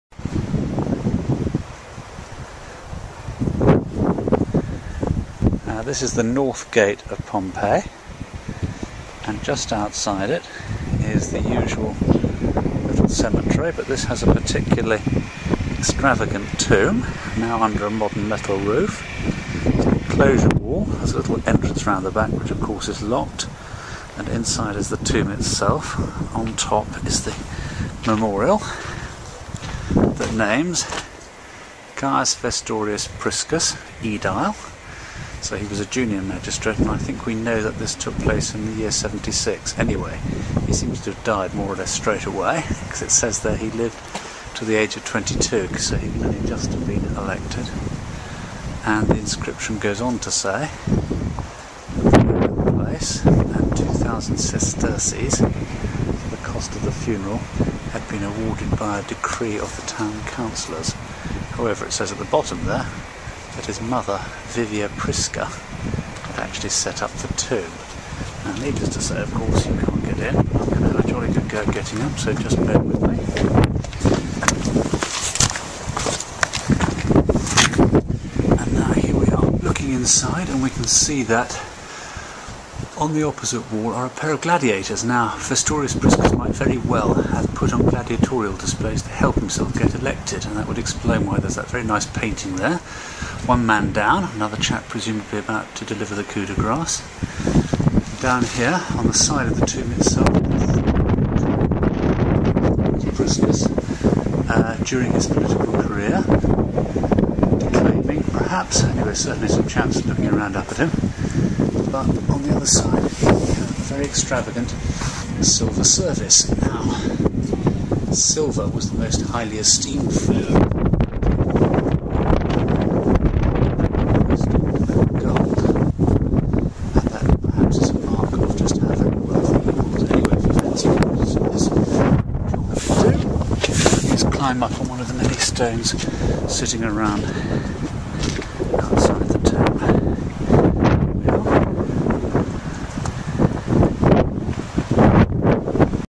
Tomb of Caius Vestorius Priscus, Pompeii. AD 79
This is the tomb of Caius Vestorius Priscus. He died at the age of 22 after being elected aedile (junior magistrate) at Pompeii in or around AD 76. His mother (Mulvia Prisca) paid for the elaborate tomb outside the Porta del Vesuvio, on a plot supplied by the local council which also paid out 2000 sesterces for the funeral. I may have mispronounced the mother's name on the video - it was difficult to read at the time and I was balancing on a stone! This young man's political career was cut off as soon as it started, though the eruption in 79 would have wrecked it anyway. The tomb has some elaborate wall-paintings inside of a gladiatorial scene (perhaps commemorating a games put on by Priscus to encourage voters to support him) and a silver service.